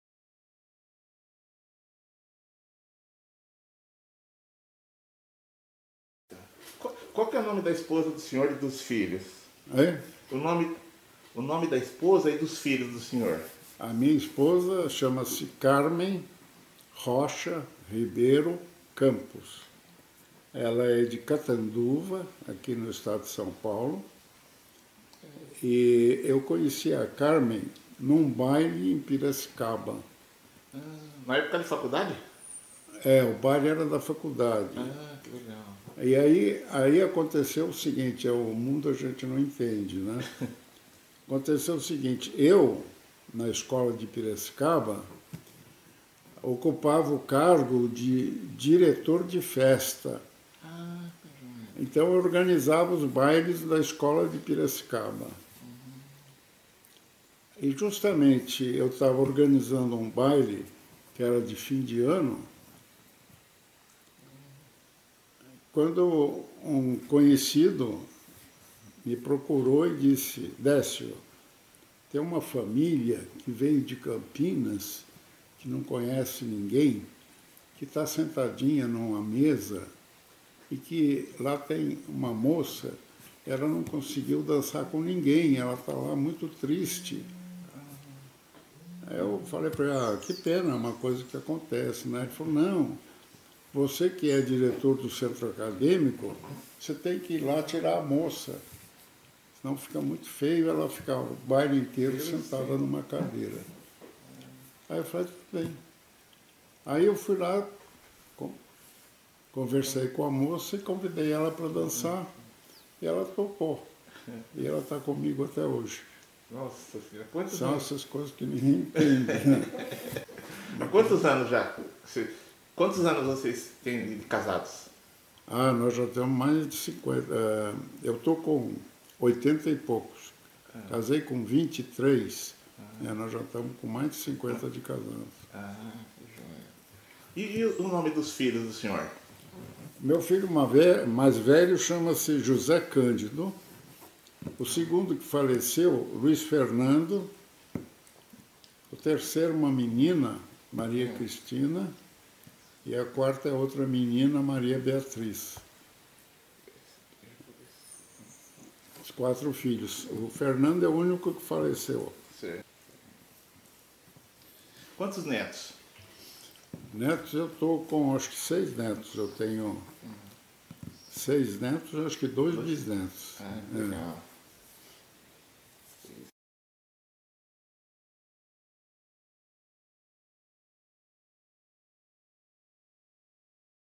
O baile da faculdade que deu origem a família
Dona Carmem Rocha Ribeiro Campos a esposa e companheira de uma vida toda
Décio Malta Campos está no Projeto "A minha história e a cidade"